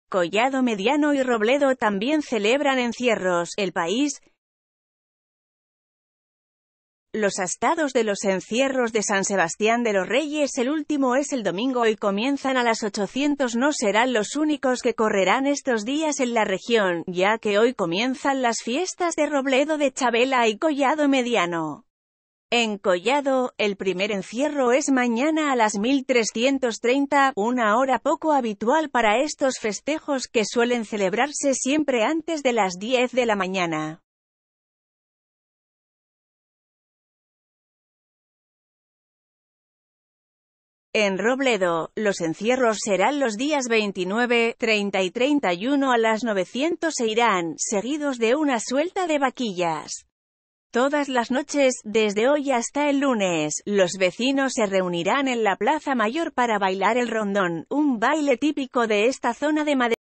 Collado mediano y robledo tambien celebran encierros (elpais)
Los astados de los encierros de san sebastián de los reyes -el último es el domingo y comienzan a las 800- no serán los únicos que correrán estos días en la región, ya que hoy comienzan las fiestas de robledo de chavela y collado mediano. En collado, el primer encierro es mañana a las 1330, una hora poco habitual para estos festejos, que suelen celebrarse siempre antes de las diez de la mañana. En robledo, los encierros serán los días 29, 30 y 31 a las 900 e irán seguidos de una suelta de vaquillas. Todas las noches, desde hoy hasta el lunes, los vecinos se reunirán en la plaza mayor para bailar el rondón, un baile típico de esta zona de madrid.